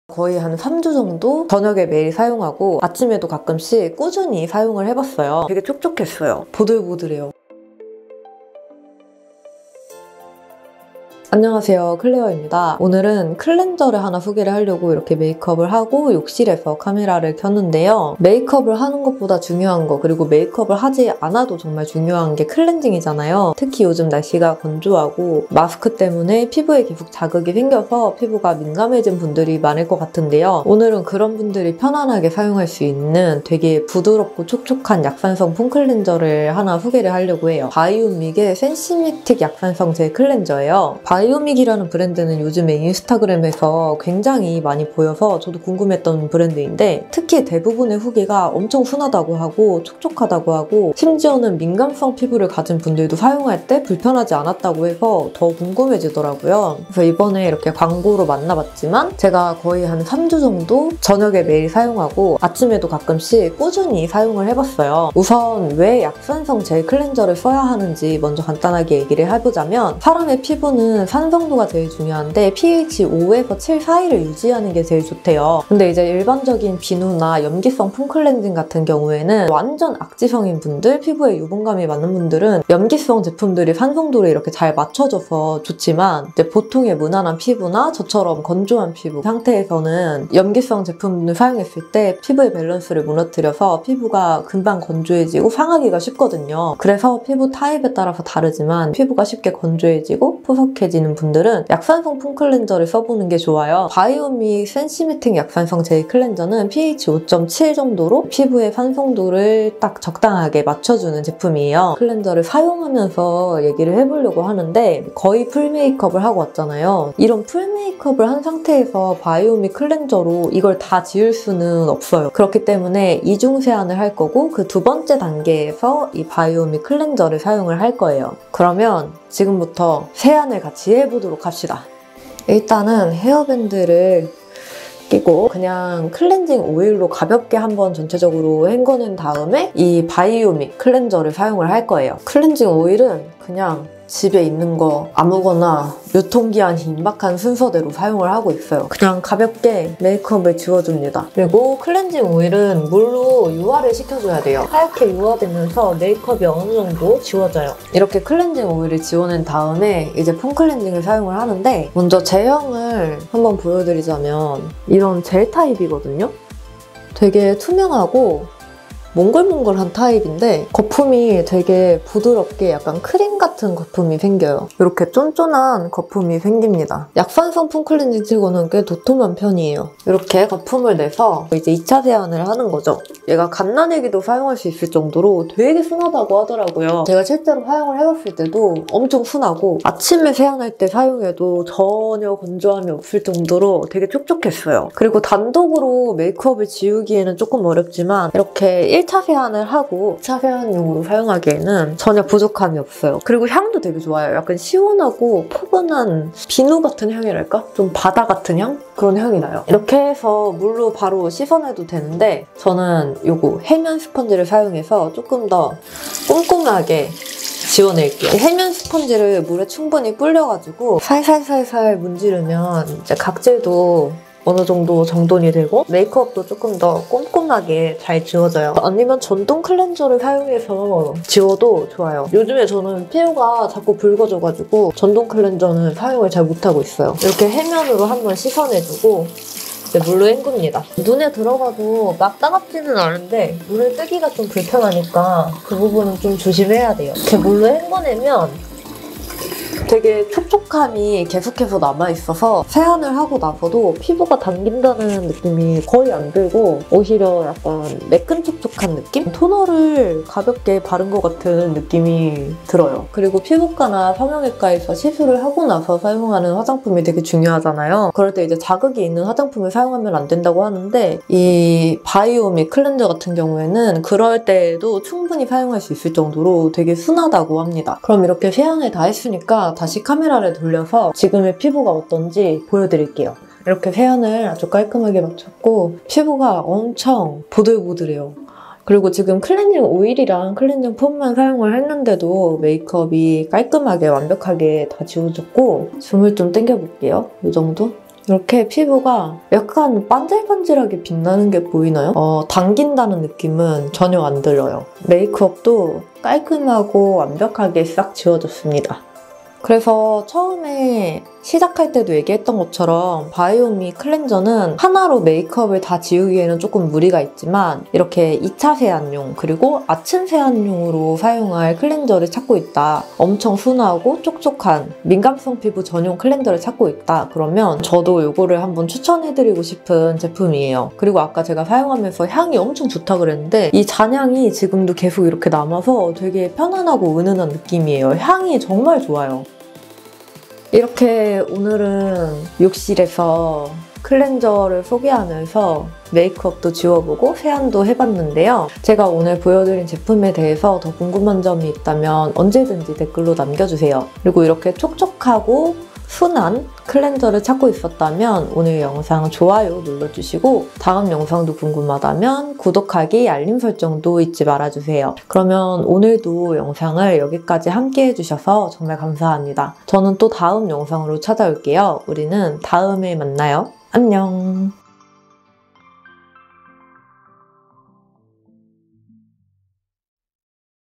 [ENG] 피부가 민감해져도 세안은 해야하니까..촉촉하고 순한 바이오믹 약산성 클렌저 | gel cleanser for sensitive skin | 클레어
바이오믹 센시메틱 약산성 젤 클렌저
BIOMEEK SENSIMETIC MILD ACID GEL CLEANSER
Retail Price: 17,000원 용량: 120ml / 4.5fl.oz
pH Balanced / pH5.7 / Gel Type